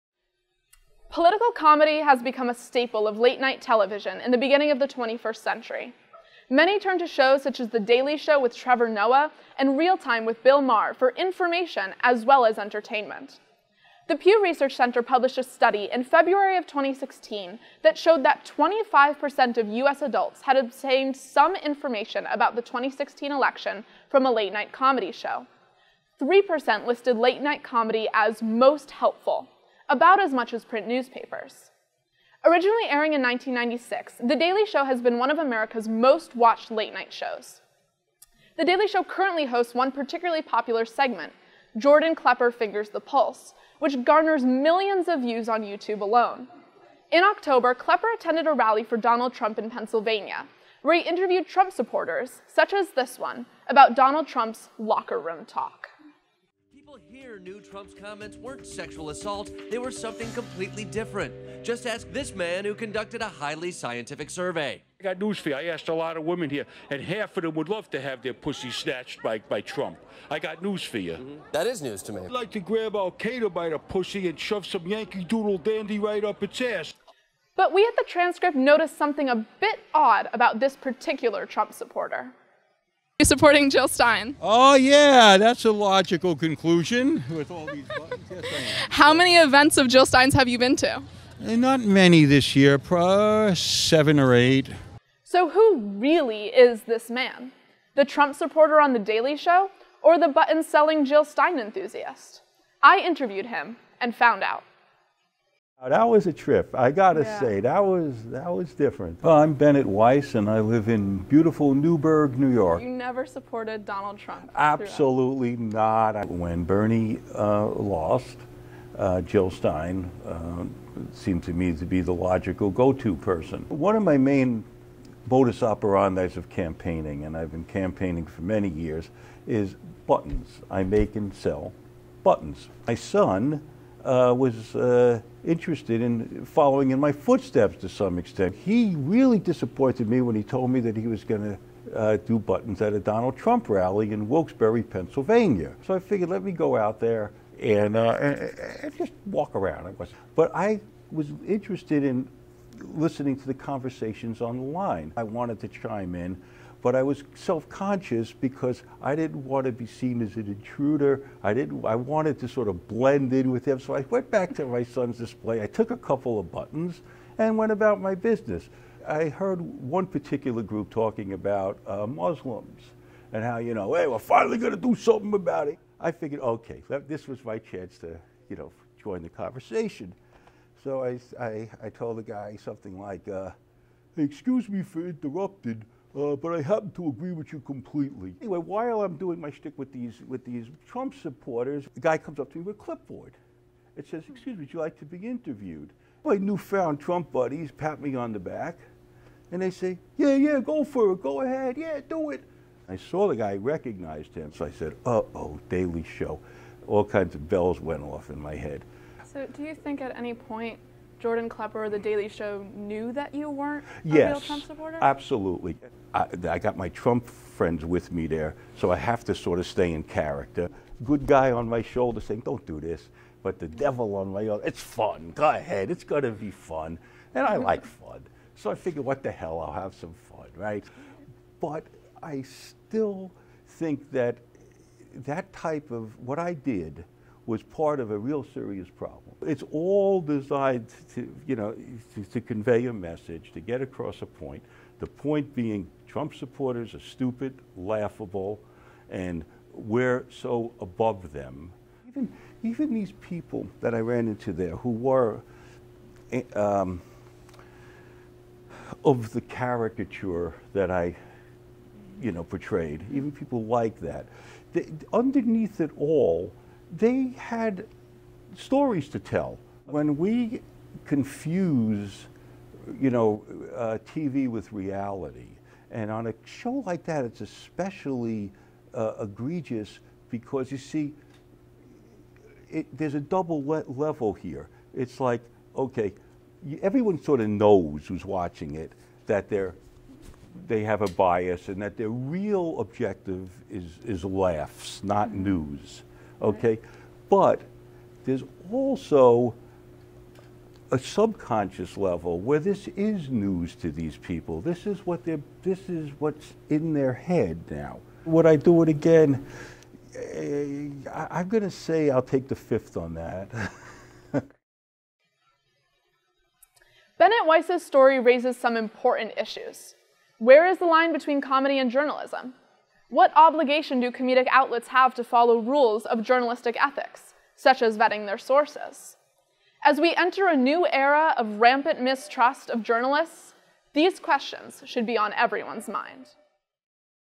Who is Bennett Weiss? - The Transcript S02 E01
Elena Frogameni examines the role of modern political comedy.